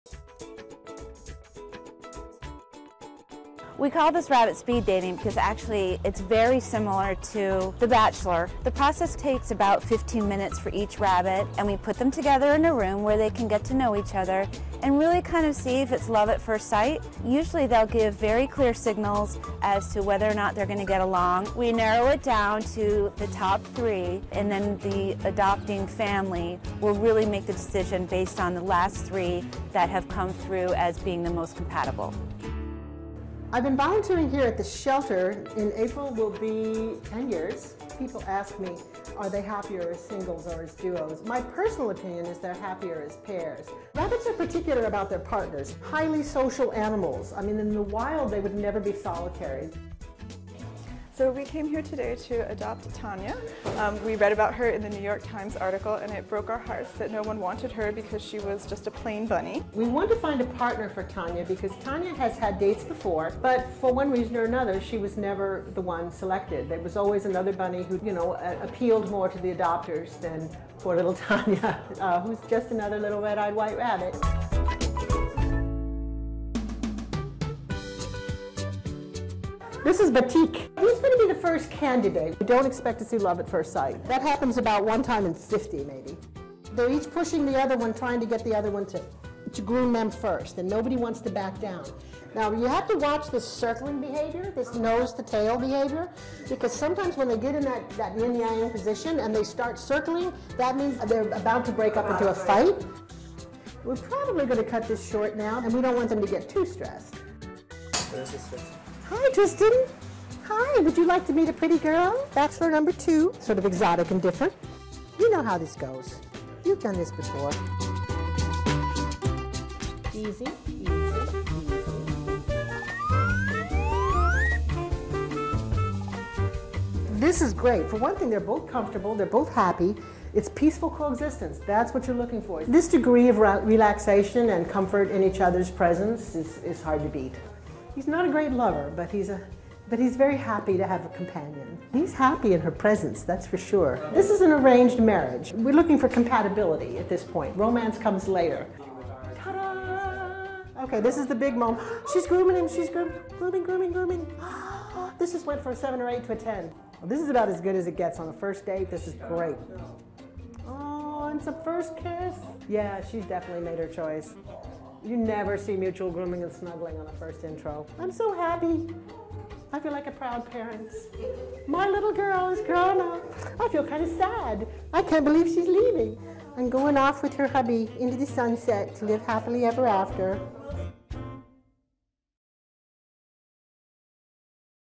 Rabbit Speed Dating Leads to Bunny Romance
A round of speed dating produces a partner for one lonely rabbit.